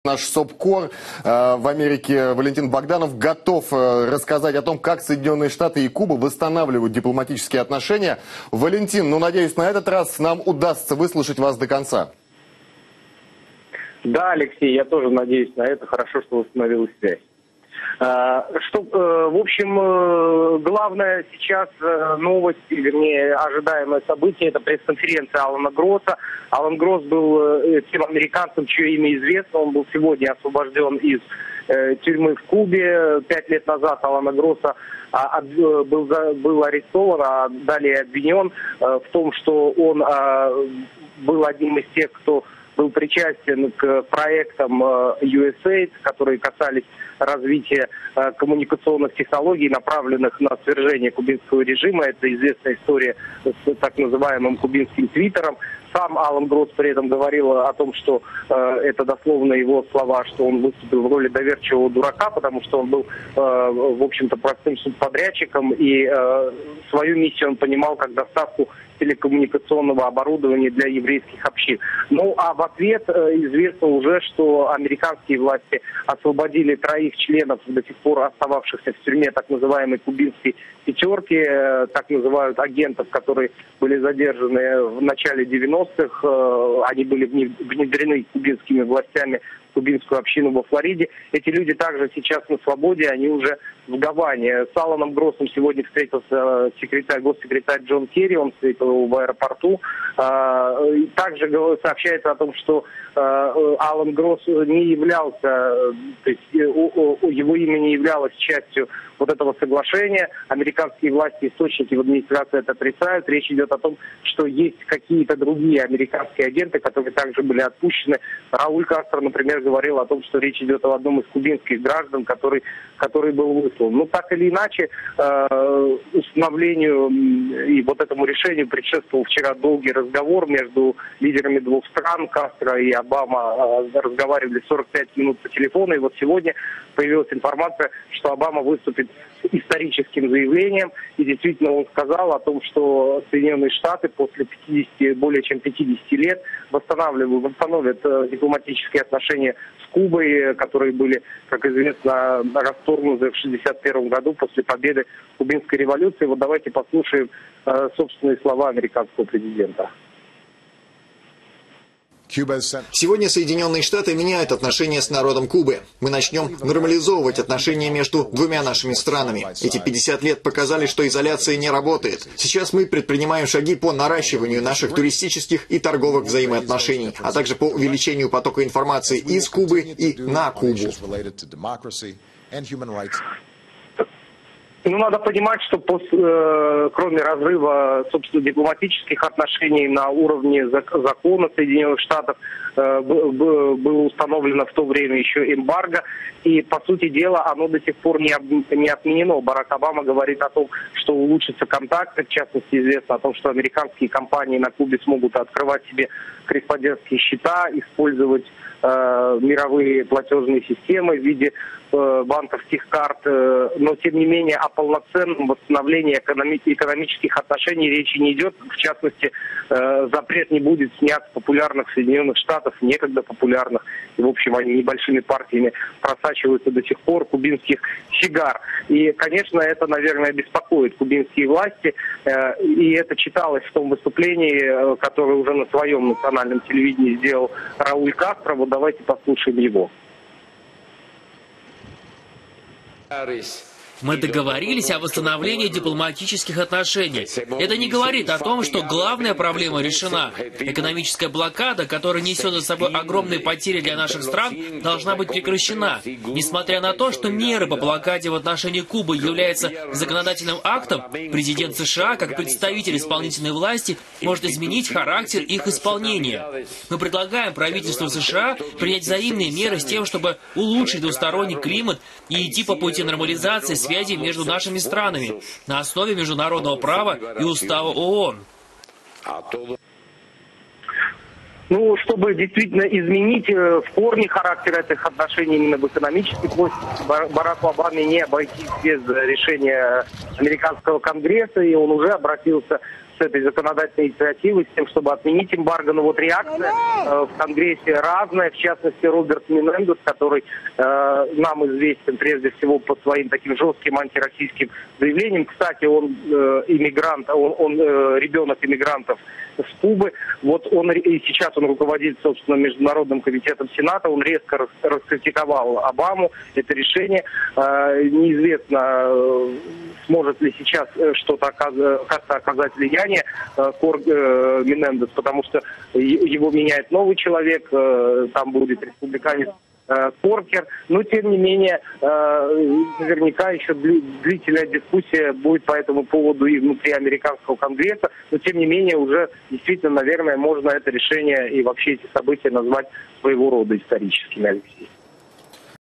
США и Куба восстанавливают отношения
Обама объявил о начале нормализации отношений США с Кубой. "Сегодня США меняют отношения с народом Кубы. Мы начнем нормализовать отношения между двумя нашими странами", — сказал президент США в специальном заявлении, которое транслировалось американскими телеканалами.